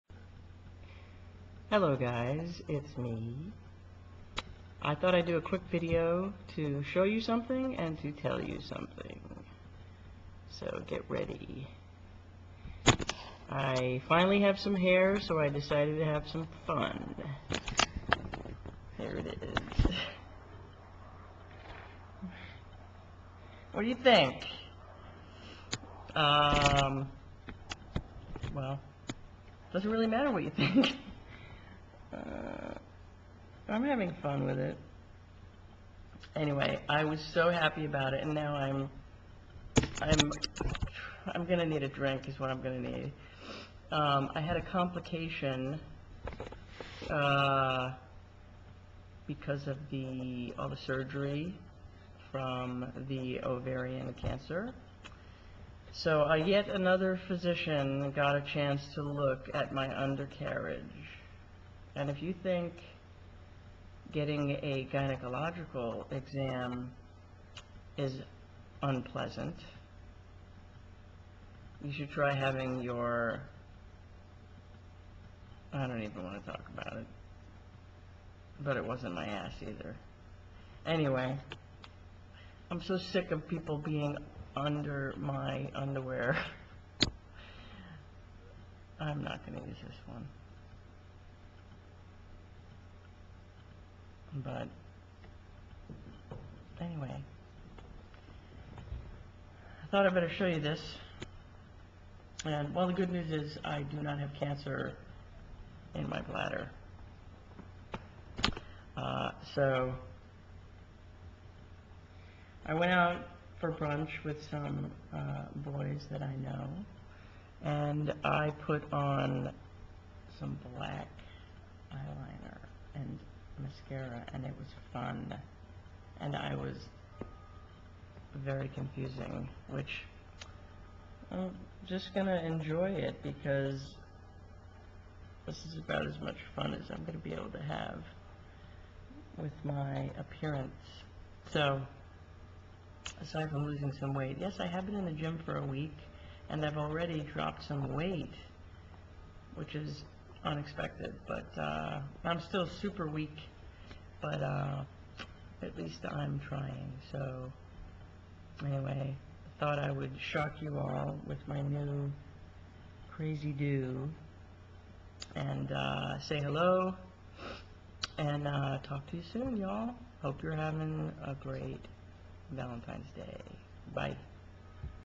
New Doo!
Now that I have some hair, I wanted to have some fun!